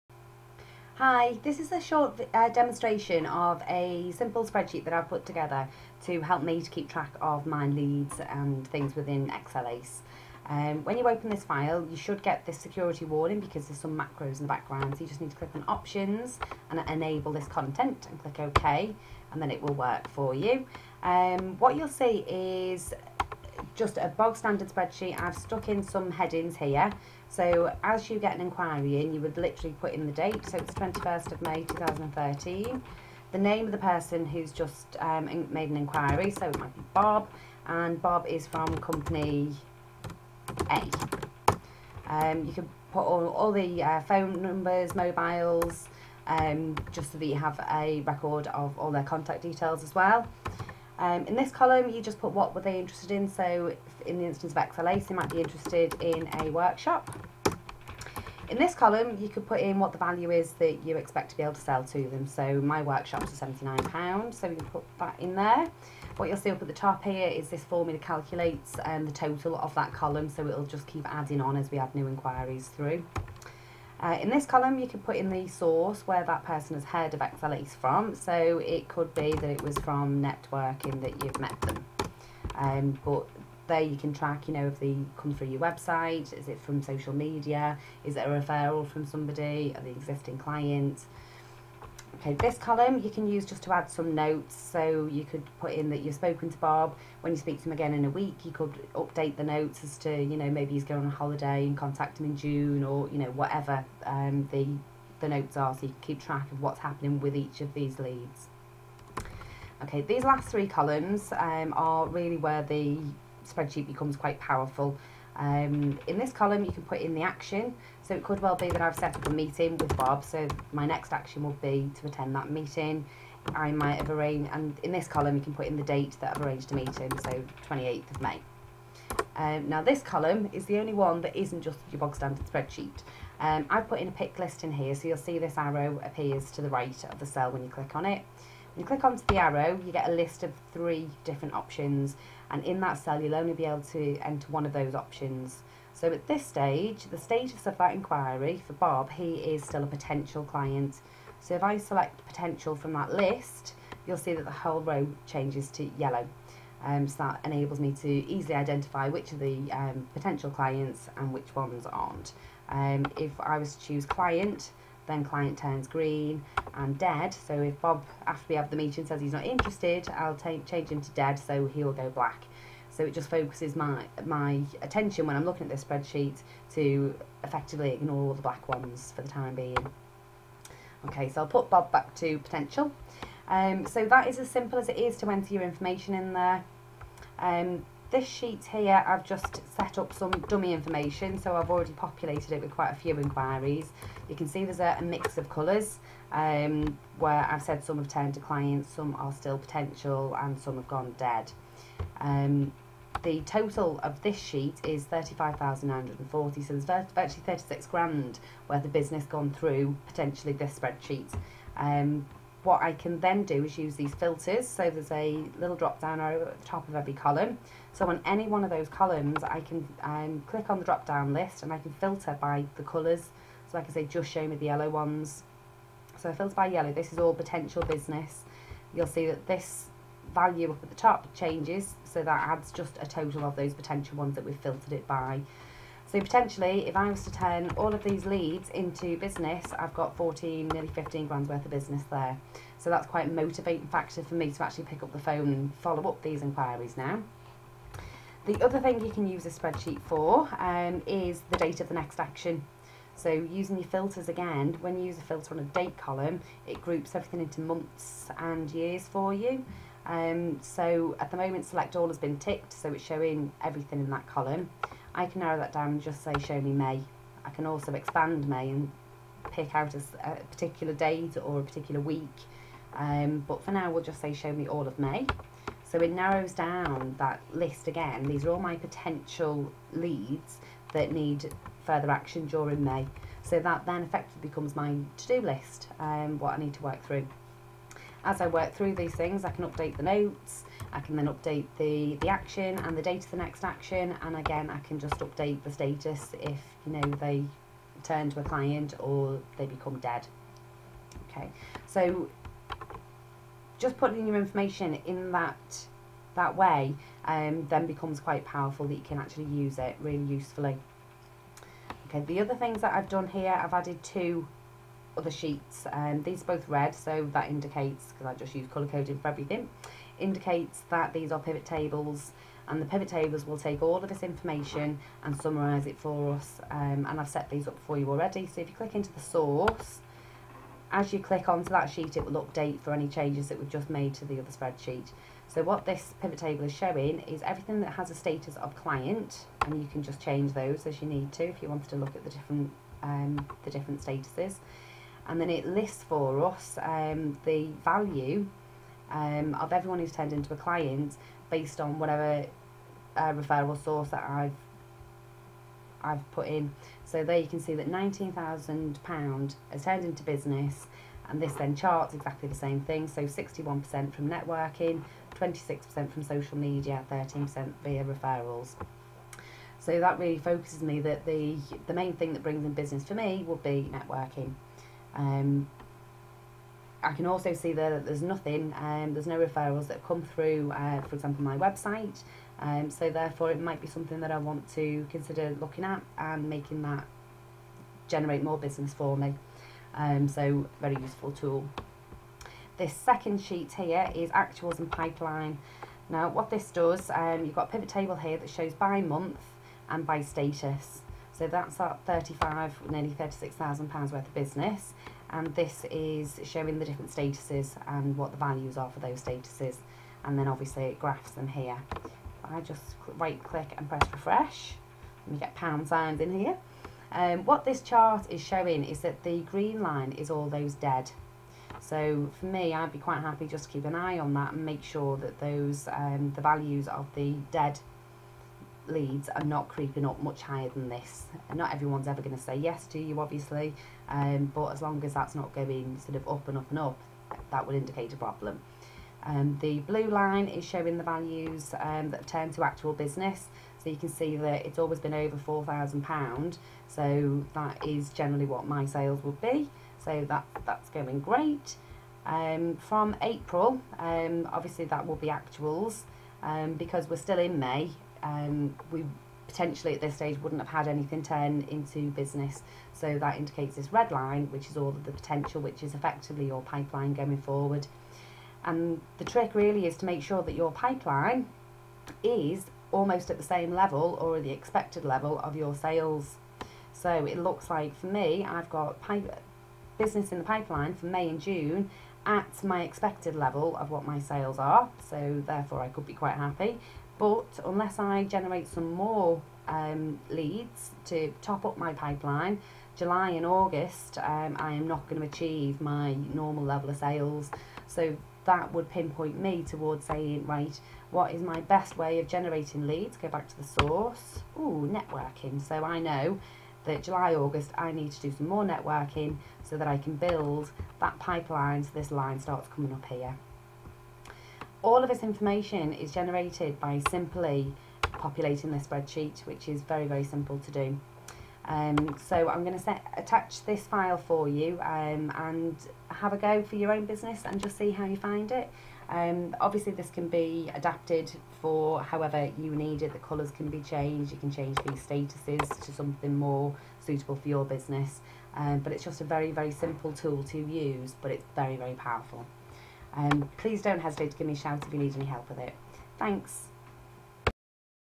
ACE Lead Tracker
This is a demo of a Spreadsheet that can be used to track Business Leads, create a To-Do List and see your future Pipeline of business!!
Super Simple yet Super Effective!!
Go to this website to get your FREE copy of the Template